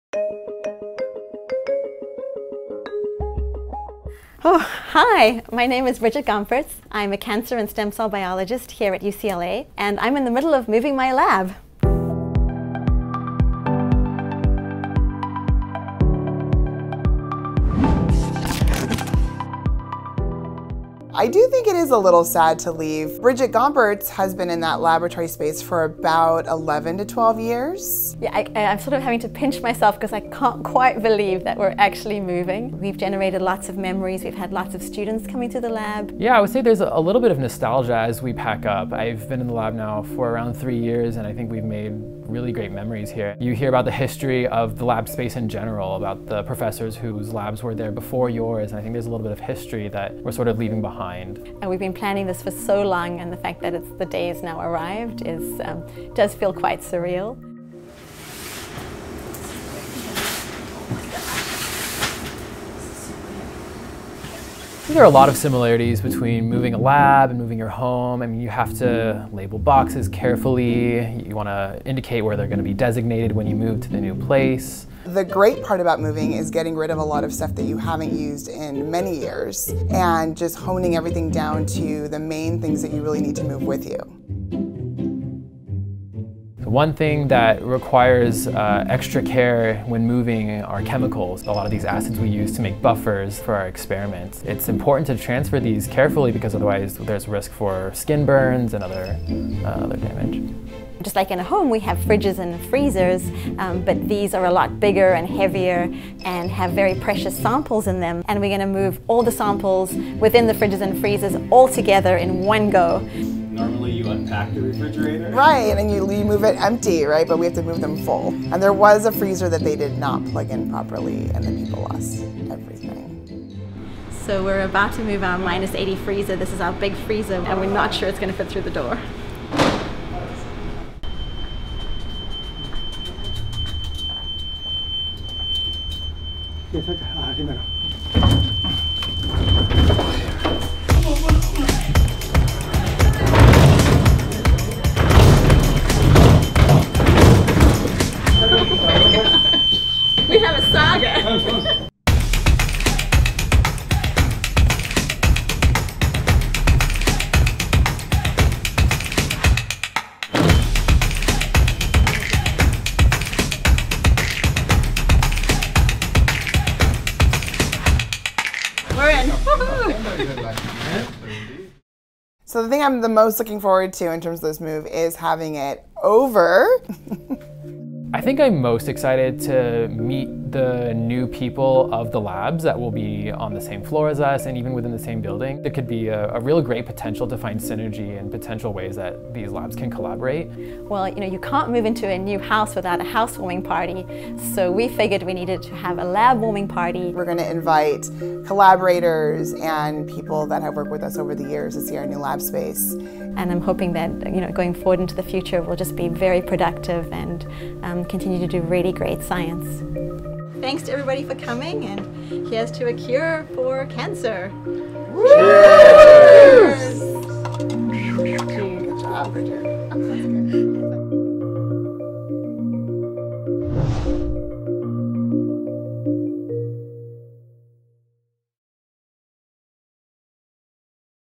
How to Move a Science Lab
If you thought moving a house was hard, try packing up an entire science lab!

Follow Dr. Brigitte Gomperts of UCLA's Jonsson Comprehensive Cancer Center as she relocates her laboratory while trying to keep her important cancer research projects running smooth.

To learn more about the latest innovations in cancer research go